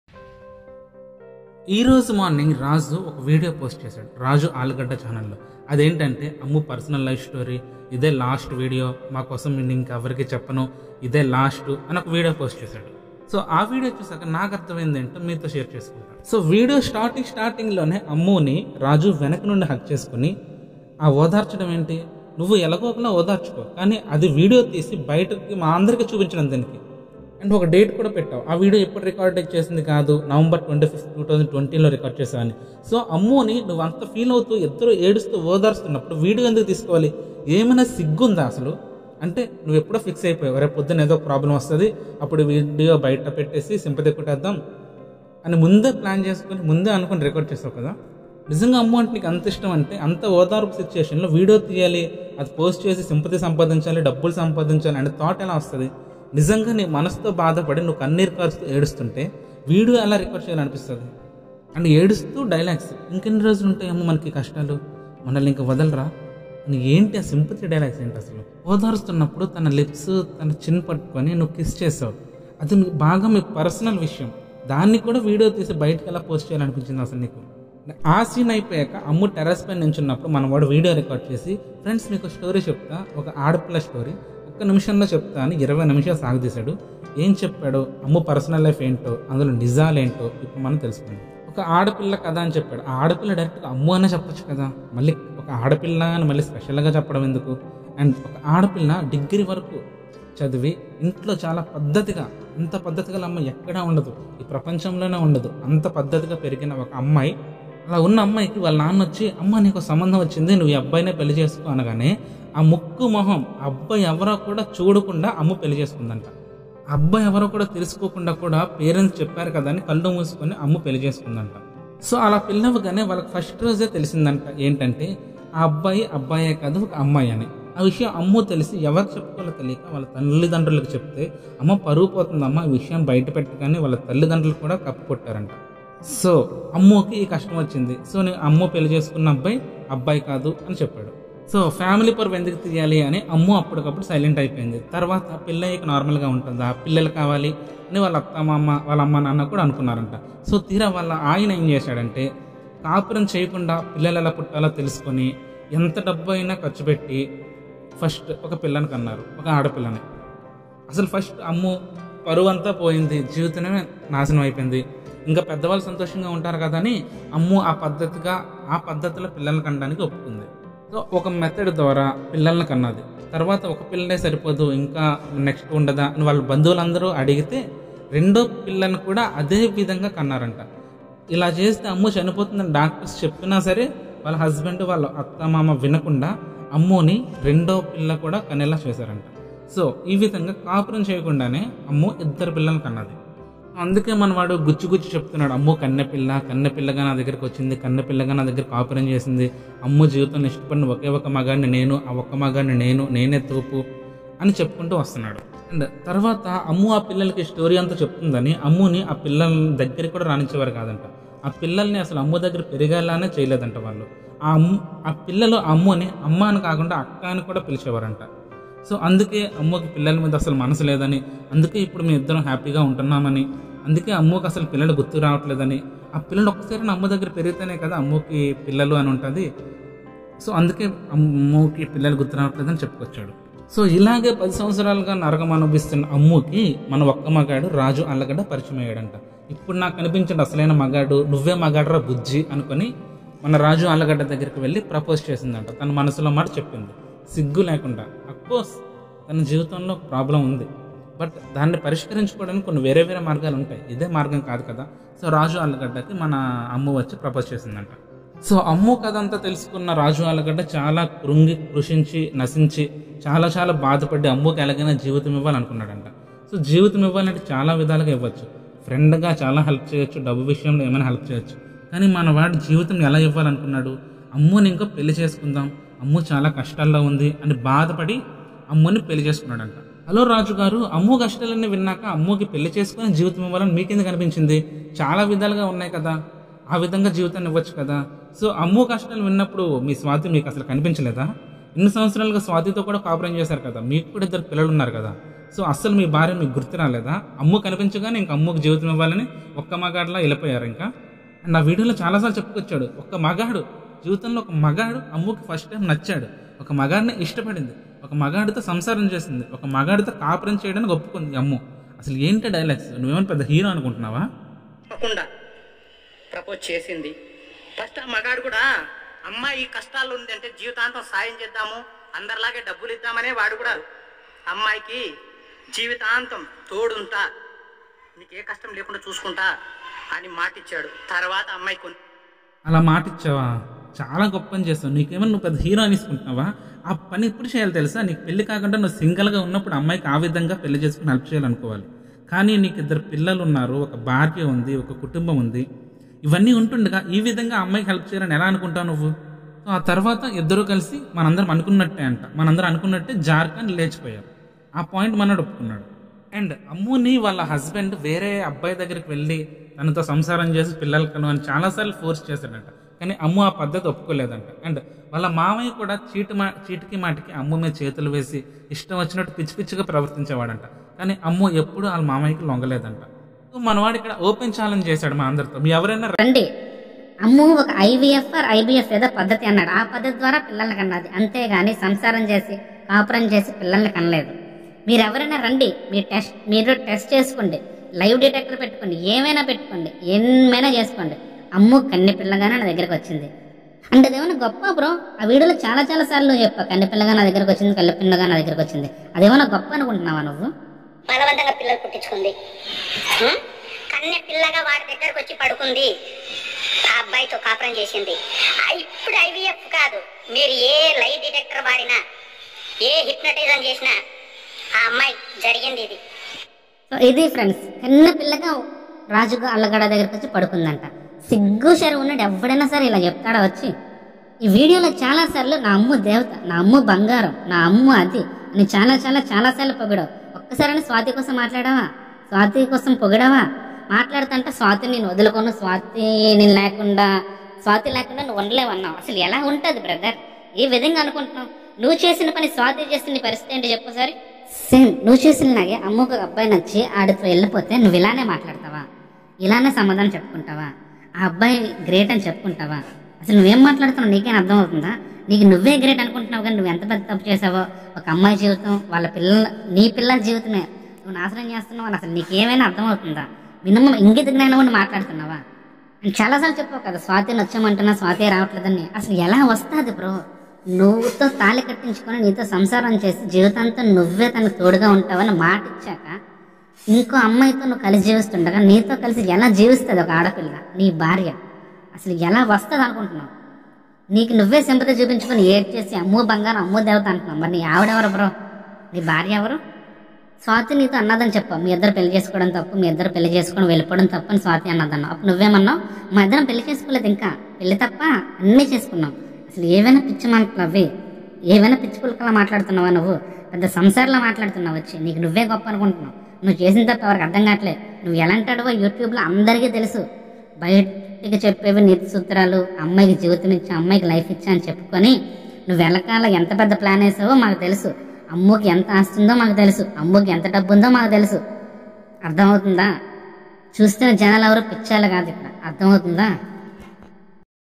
జనాల్ని పిచోళ్లు అనుకోని కొత్త కధలు చెప్తున్నాడు | Raju Allagadda | Ammu Vlogs | రాజు కి అరెస్ట్
రాజు కి అరెస్ట్ వారంట్ రెడీ కట్టు కధలు అల్లాడం లో నెంబర్.1 జనాలను పిచ్చివాలను చేస్తున్నాడు